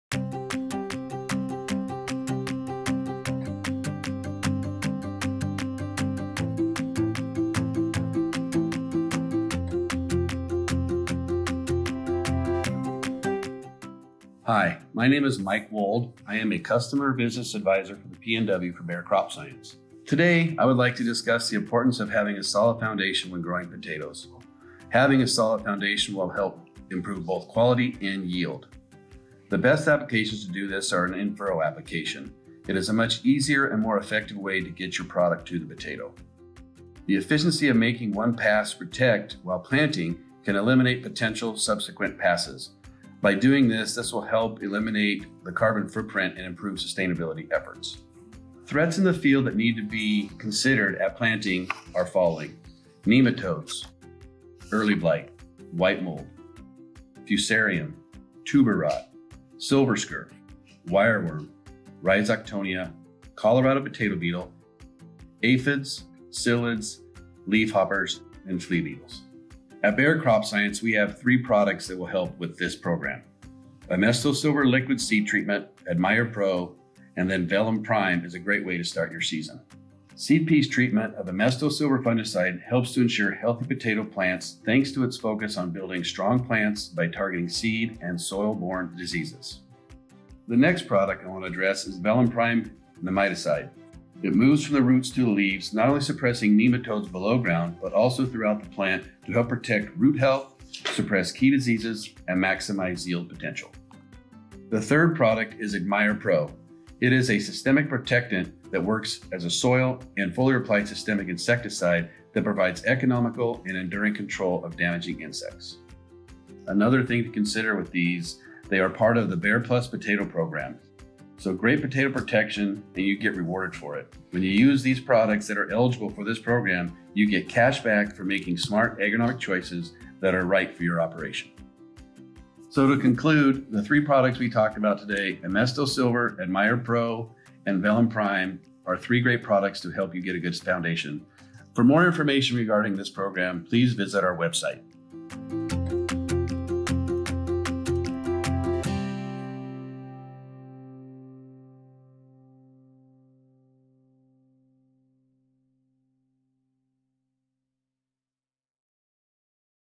A FORCE IN AT-PLANT SOLUTIONS
Prevention is key with potato crops and it starts at planting.  Learn about the threats that should be considered when making potato planting decisions and how the Bayer PLUS Potato eligible crop protection products within the 75-Day IPM program are a force in at-plant solutions. Products featured in this video are Admire Pro insecticide, Emesto Silver seed treatment and Velum Prime fungicide/nematide.